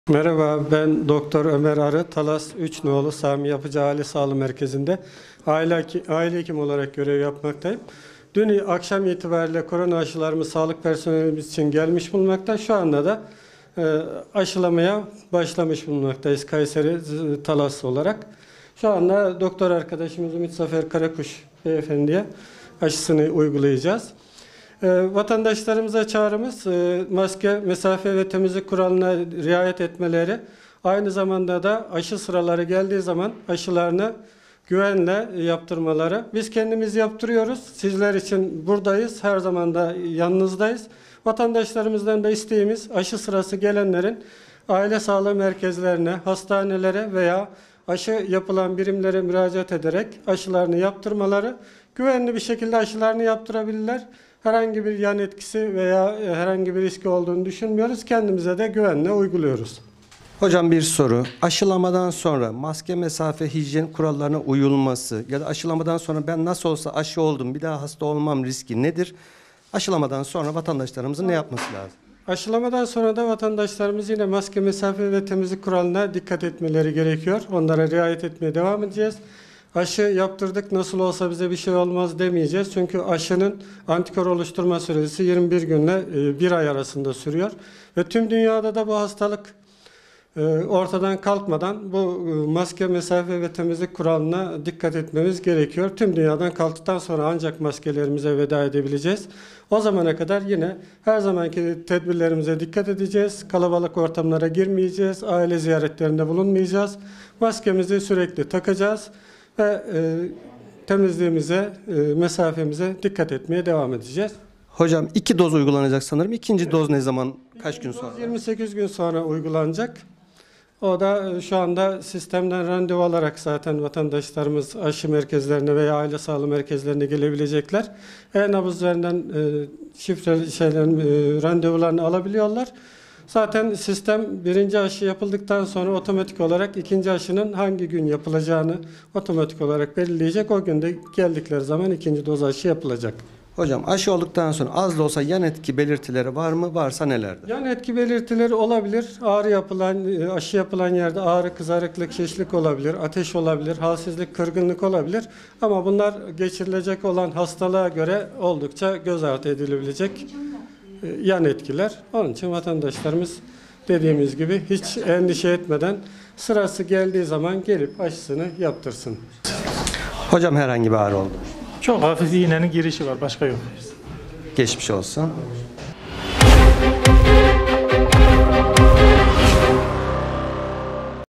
Kayseri'de İlk Aşı Uygulandı CANLI YAYINDA İLK AŞILANMA
Talas 3 nolu Sami Yapıcı Aile Sağlığı Merkezinde ilk aşı uygulaması yapıldı. Aile Hekimi Doktor Ömer Arı vatandaşları aşı konusunda bilgilendirdi. Talas 3 Nolu Sami Yapıcı Aile Sağlığı Merkezi ,Aile hekimi Ömer Arı vatandaşlara çağrıda bulundu.
"Dün akşam itibariyle koronavirüs aşıları sağlık personelimiz için geldi. Aşılamaya başlamış bulunmaktayız. Vatanşlarımıza çağrımız maske ve mesafe ve temizlik kurallarına riayet etmeleri aynı zamanda aşı sıraları geldiğinde aşılarını güvenle yaptırmaları. Biz yaptırıyoruz sizler için buradayız. Her zaman yanınızdayız. Vatandaşlarımızdan istediğimiz aşı sırası gelenlerin aile sağlığı merkezi hastane veya aşı yapan birimlere müracaat ederek aşılarını yaptırmaları. Aşılarını güvenle yaptırabilirler herhangi bir yan etkisi ve riski olduğunu düşünmüyoruz kendimize de güvenle uyguluyoruz" dedi.
"AŞILAMADAN SONRA MASKE MESAFE VE TEMİZLİĞE DİKKAT"
Maskeleri çıkarmayı merakla bekleyen vatandaşlarımız için Ömer Arı "Aşılamadan sonra vatandaşlarımızın yine maske mesafe ve temizli kurallarına dikkat etmesi gerekiyor. Aşı yaptırdık nasılsa bize birşey olmaz demeyeceğiz çünkü aşınların antikor üretme süresi 21 gün 1 ay sürüyor. Tüm dünyada bu hastalık ortadan kalkmadan maske mesafe temizlik kuralına dikkat edeceğiz. Kalabalık ortamlara girmeyeceğiz. Aile ziyaretlerinde bulunmayacağız. Maskemizi sürekli takacağız. Temizliğimize dikkat edeceğiz. " dedi

İkinci dozun 28 gün sonra uygulanacağını söyleyen arı vatandaşların sistemden randevu alarak aşı merkezlerine ve aile sağlığı merkezlerine gelebileceğini söyledi. "E nabız üzerinden randevu alabilirsiniz.
Sistem ilk aşıdan sonra otomatik olarak ikinci aşının ne zaman yapılacağını belirleyecek" dedi.

"KORONAVİRÜSE GÖRE YAN ETKİLER GÖZ ARDI EDİLEBİLİR "

"Aşı sonrası yan etki belirtiler olabilir aşı yapılan yerde ağrı, kızarıklık, şişlik olabilir. Ateş, halsizlik, kırgınlık olabilir ama bunlar geçirilecek olan hastalığa göre göz ardı edilebilecek yan etkiler. Vatandaşlarımız endişe etmesin sırası geldiği zaman gelip aşıyı yaptırsın" dedi.